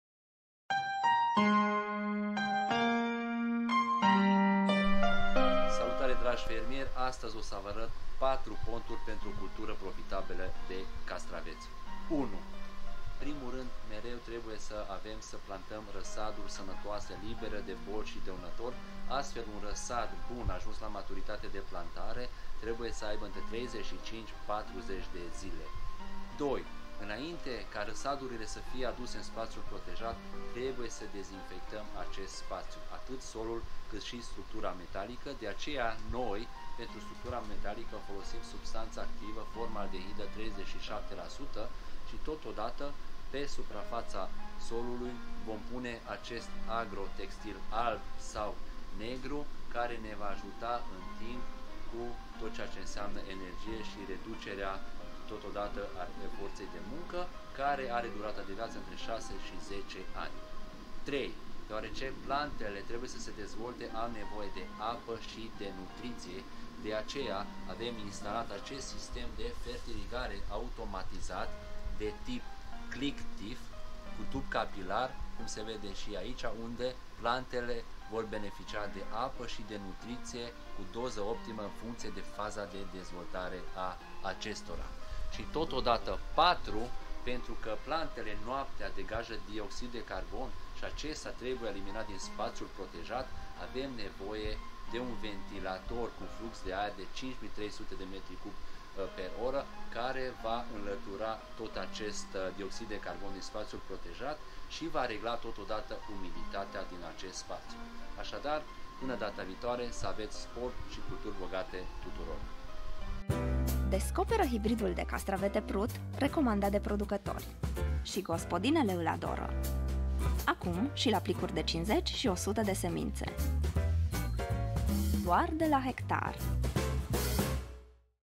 4 ponturi pentru culturi profitabile
Câteva sfaturi utile pentru o cultură profitabilă de castraveți!

Pentru o reușită garantată, recomandăm hibrizii de castraveți românești din portofoliul Hektar - PRUT F1, TISA F1 și DORNA F1.

1. Plantarea unui răsad sănătos, liber de boli și dăunători, de 35-40 de zile
2. Dezinfectarea structurii metalice cu substanța activă - formaldehidă 37%, folosire agrotextil alb sau negru pe sol, care are o durată de viață 6-10 ani
3. Instalare sistem de fertirigare de tip CLICKTIF cu tub capilar
4. Instalare ventilator cu un flux de aer de 5300 m3/oră pentru reglarea umidității din spațiul protejat

Pentru mai multe detalii accesați pagina noastra online - hektar.ro